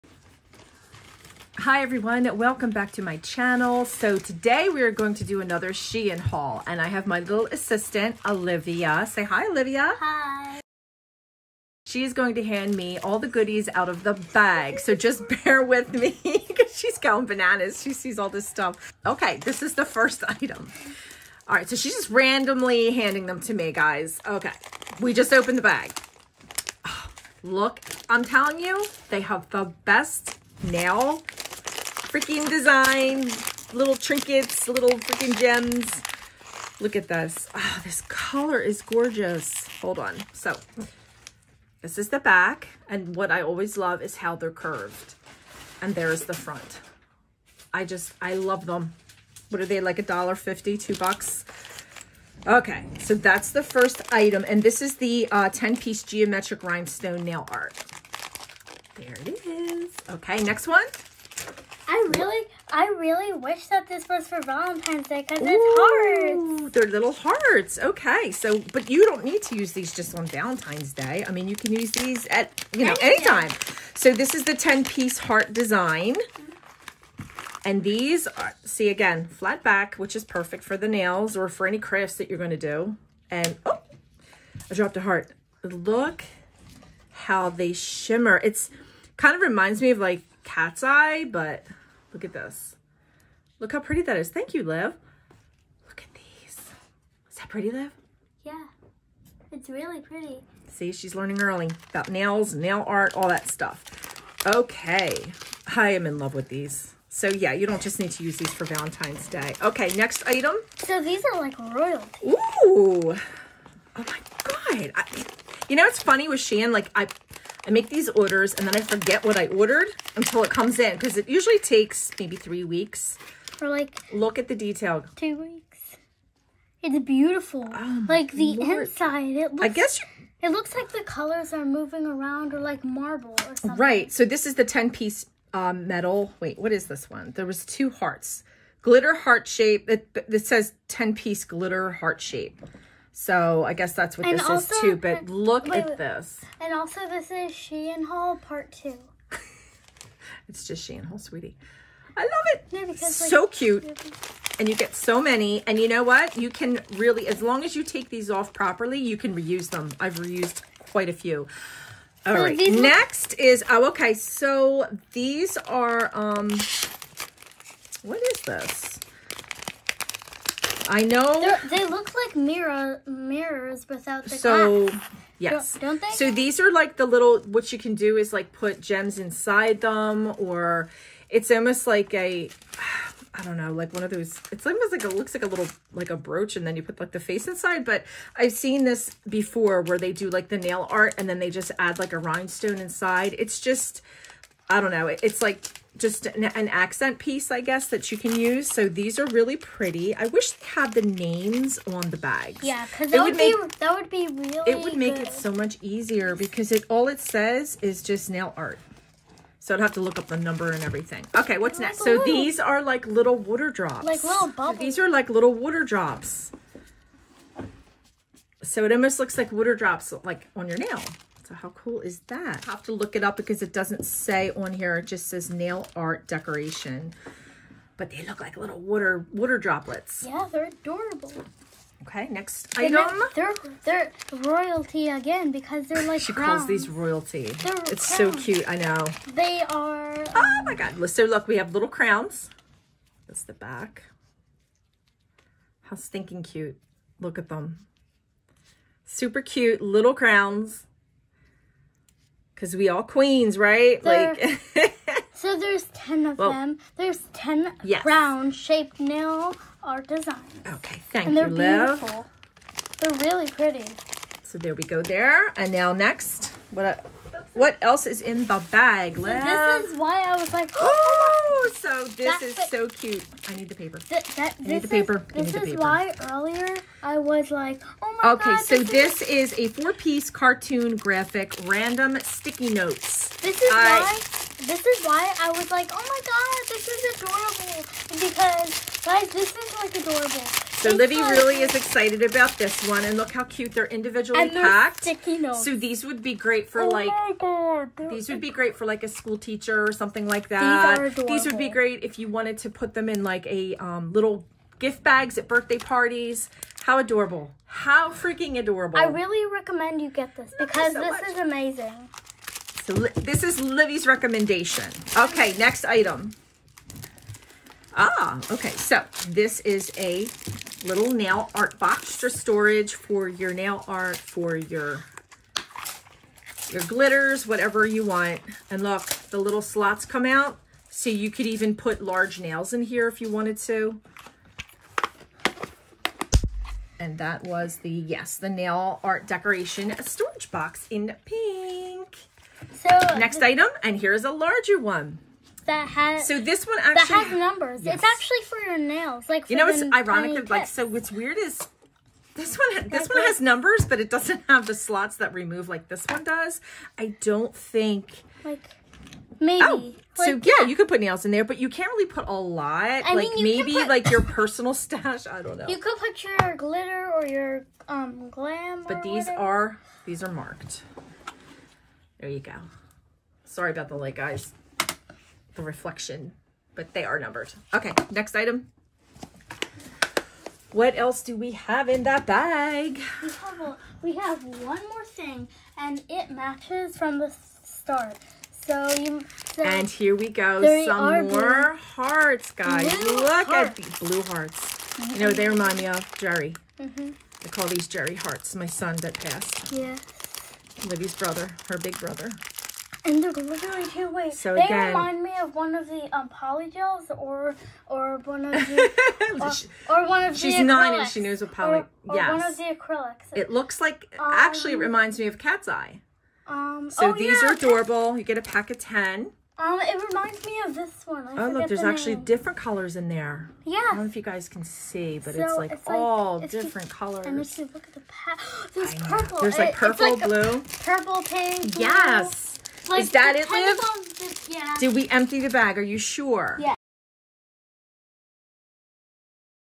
SHEIN Haul with Livi 🥰
Hello everyone, been mia for a bit.  Both my daughter and I have covid and its been a rough last 5 days especially for me.  🤒😷🤧 So glad I recorded this before we got sick.  Sorry I was way off on some of the descriptions in the vid, here they are below.  Hopefully will be up & running soon.   Enjoy!

⬇️Products Mentioned:
4pc Cartoon Sticky Notes - $2.12
10pc Glitter Hearts - Red Violet - $1.00
10 pc Glitter Hearts - Blue - $1.00
10 pc Geometric Rhinestone - $2.50
10pc Crown Shaped Rhinestone - $2.00
10pc Heart Design/Gold/Pearl - $2.50
10pc Metal Nail Art (Oval)  - $1.00
1pk Galaxy Nail Art ( the drops) mint blue - $1.00
Clear numbered storage box - $1.50
Pink storage box - $1.00

Please click on my LinkTree 🌲